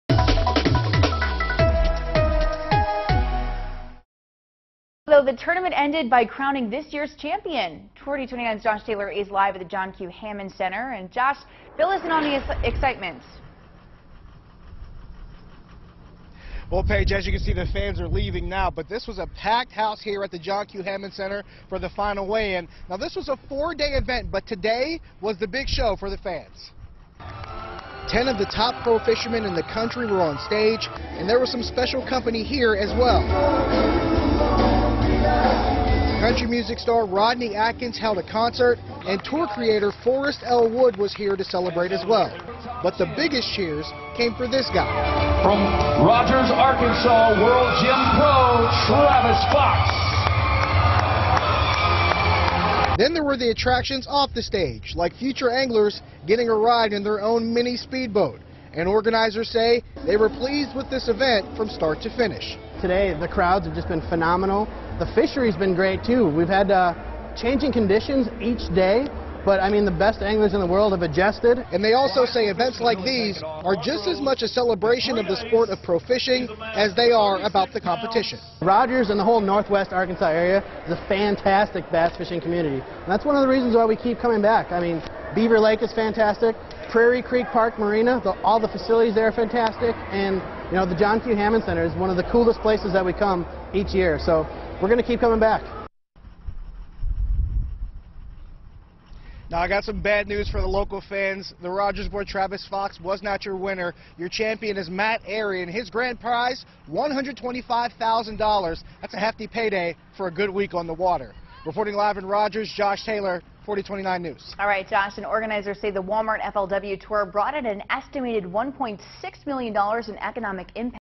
FLW Tournament Champion Crowned
4029s Josh Taylor has a look at the final weigh in, and tells us who won this year's FLW Tournament.